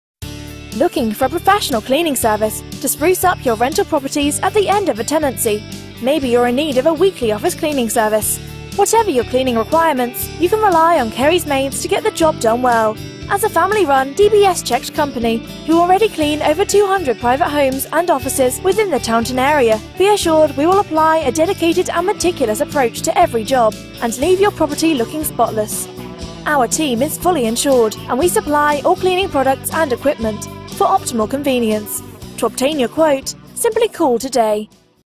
Professional Cleaning - Kerrie's Maids
Whatever your cleaning requirements, you can rely on Kerrie's Maids to get the job done well. As a family-run, DBS -checked company who already clean over 200 private homes and offices within the Taunton area, be assured we will apply a dedicated and meticulous approach to every job and leave your property looking spotless. Why not visit our webite today for further information?